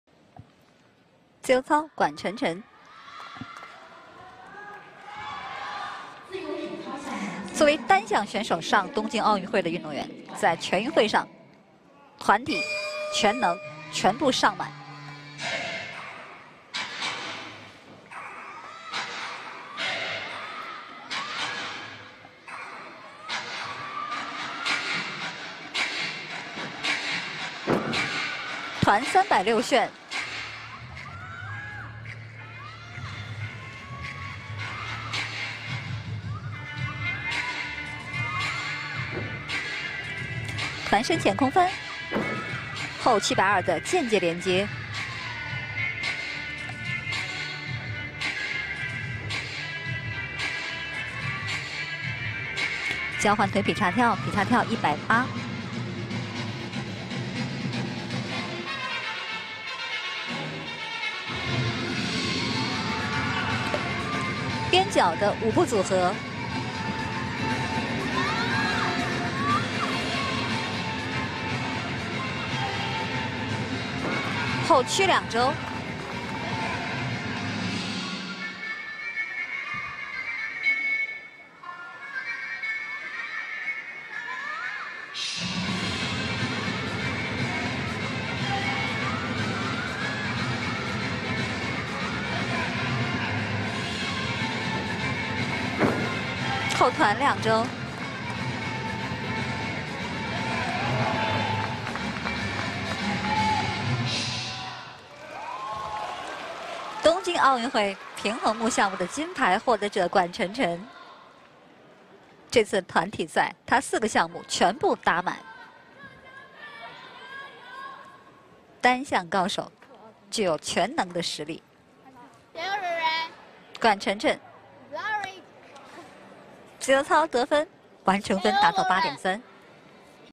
管晨辰GuanChenchen -floor exercise -Women's Gymnastics Team Final -2021 China National Games体操女子团体决赛自由操
2021全运会体操女子团体决赛——自由操——浙江队 管晨辰
2021 China National Games Women's Gymnastics Team Final—— floor exercise——Guan Chenchen from Team ZheJiang

第十四届全国运动会于2021年9月15日至2021年9月27日在陕西省举行。
The 14th National Games of the People's Republic of China will be held in Shaanxi Province from September 15, 2021 to September 27, 2021.